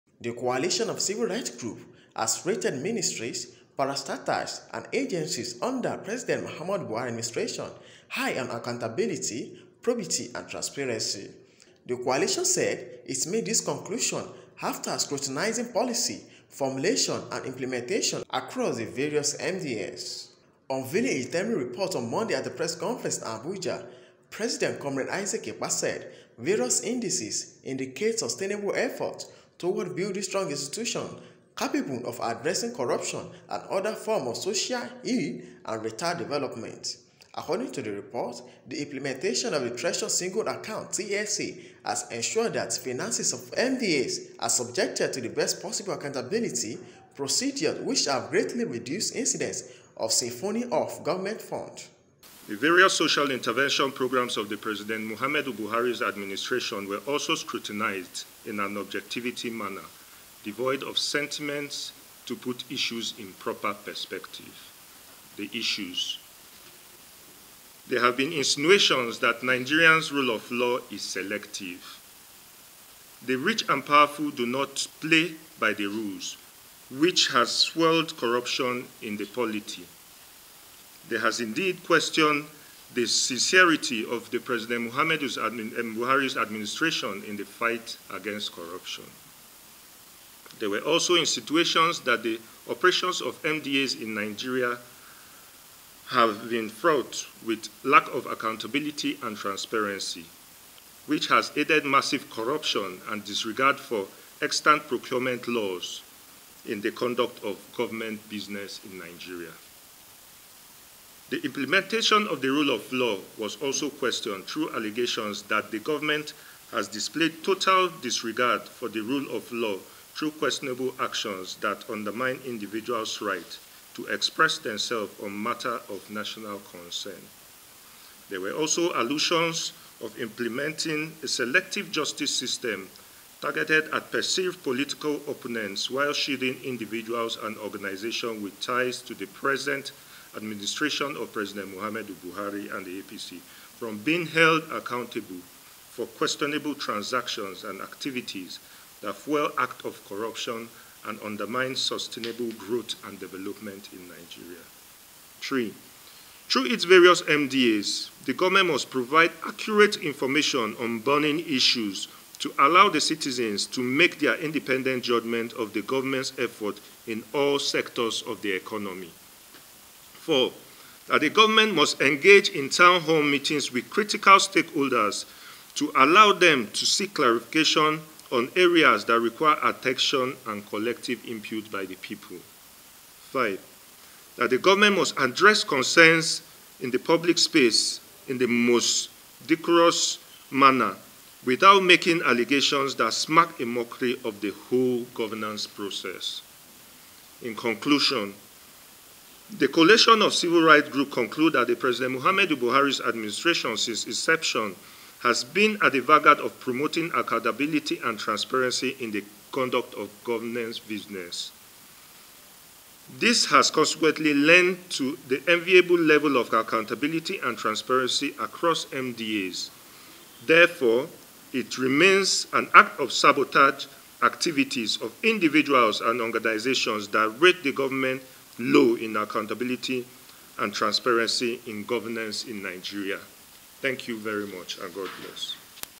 Group lauds MDAs’ Accountability, Probity Under PMB’s Administration
The Coalition of Civil Rights Groups (CCRG) has rated ministries, parastatals and agencies under President Muhammadu Buhari's administration high on accountability, probity and transparency.

The coalition said it made this conclusion after scrutinizing policy formulation and implementation across the various MDAs.